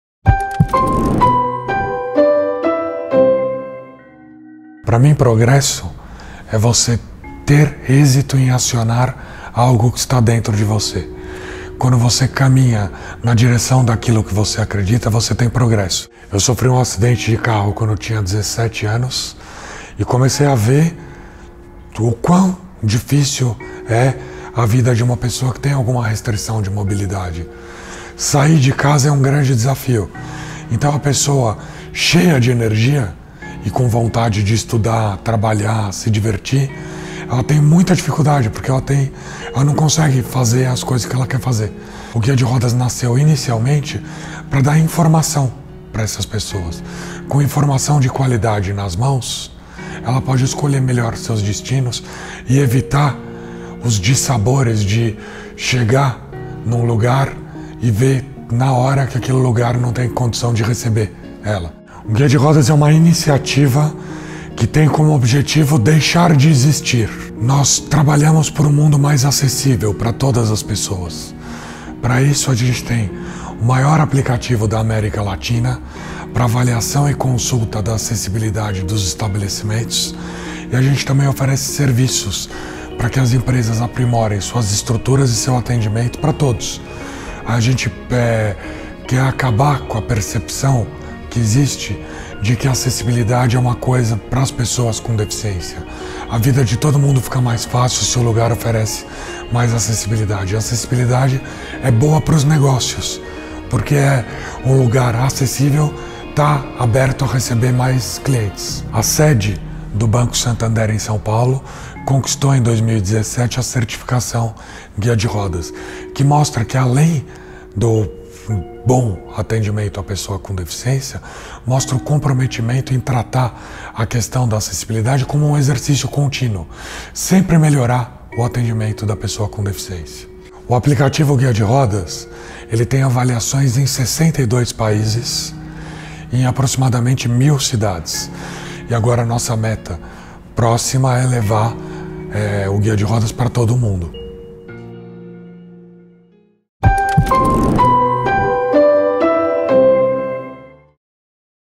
Um “app” para classificar os edifícios de acordo com seu grau de acessibilidade
Um acidente de trânsito deixou Bruno Mahfuz numa cadeira de rodas aos 17 anos. Isso também mudou sua percepção e observou a quantidade de dificuldades físicas que as pessoas com deficiência enfrentam em seu cotidiano. Agora ele criou Guiaderodas, um aplicativo que classifica os edifícios de acordo com sua acessibilidade.